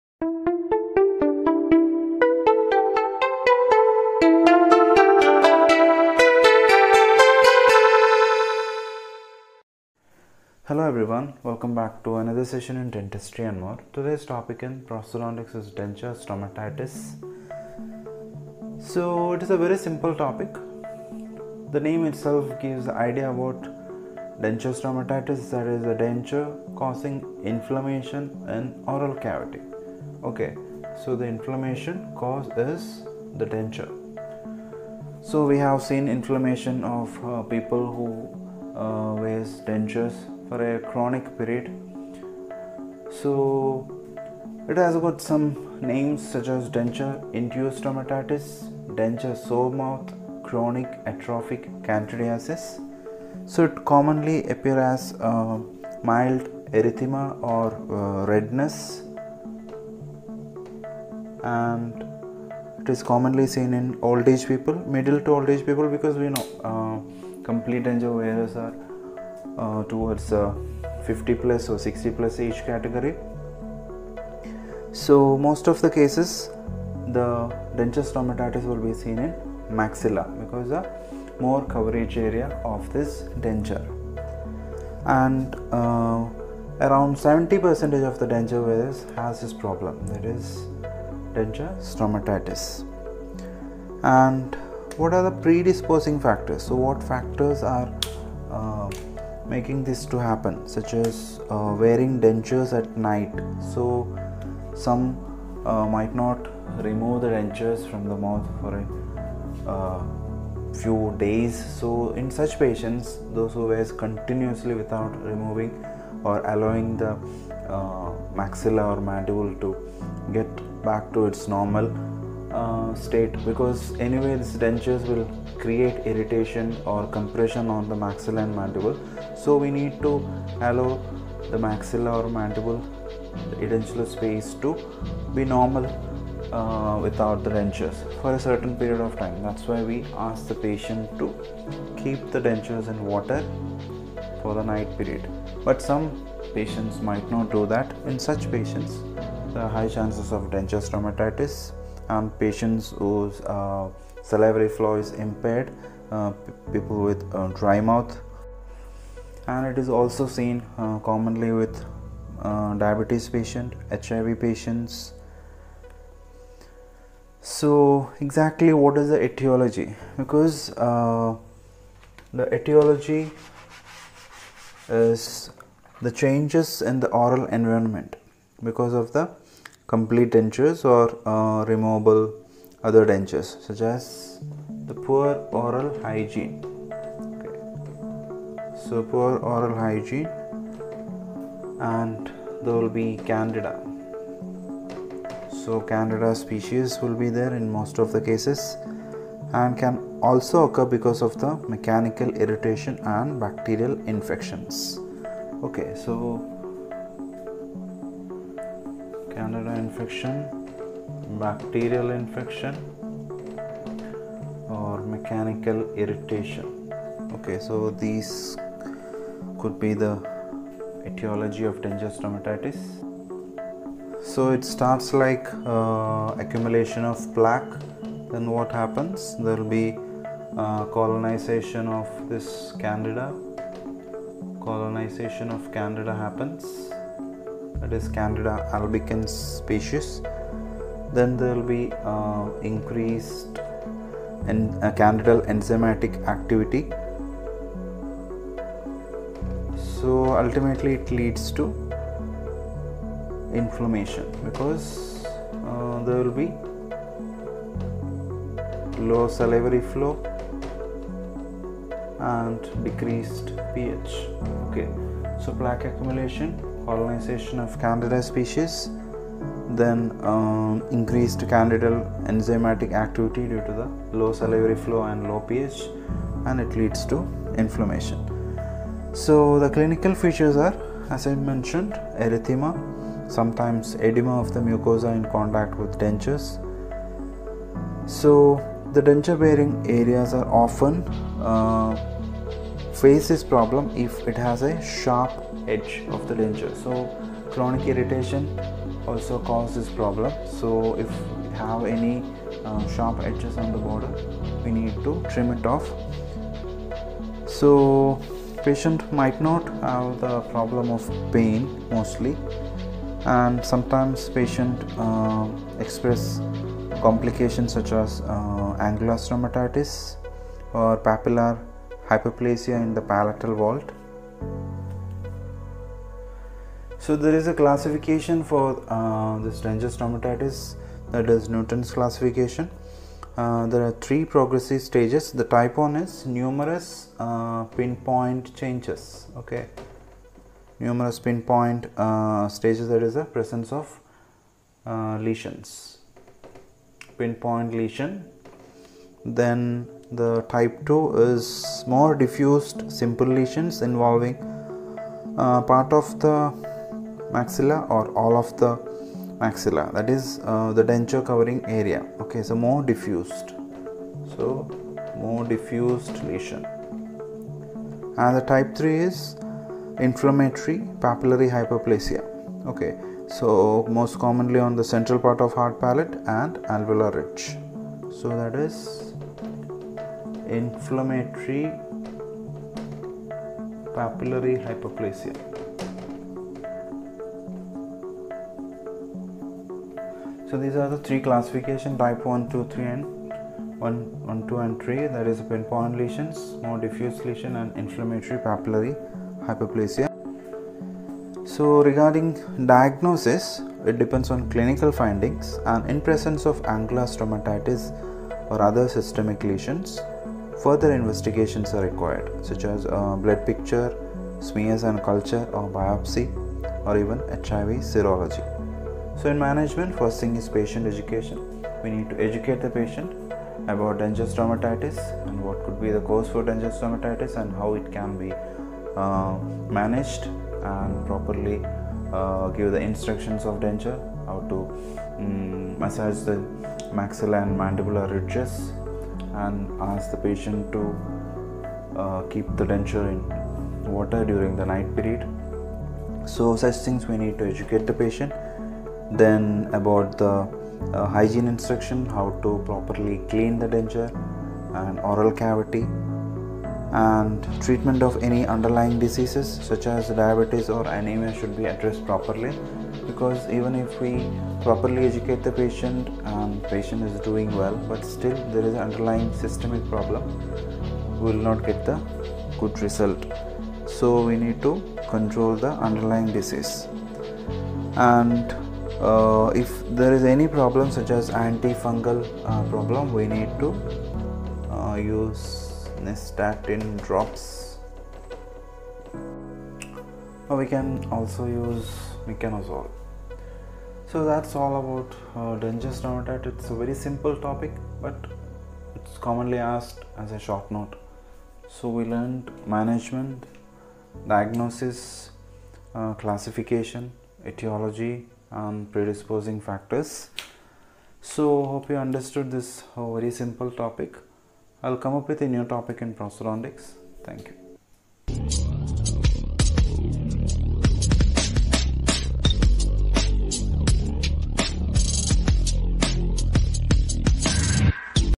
DENTURE STOMATITIS
Denture-induced stomatitis is a common inflammatory reaction related to the wearing of dentures usually involving Candida (yeast) species. Less common forms of denture stomatitis may be due to mechanical trauma or an allergic contact reaction. Stomatitis means a sore mouth.

Mic used

2)Oral medicine & Radilology

3)Oral pathology

4)Pedodontics

5)Orthodontics

6)Public health dentistry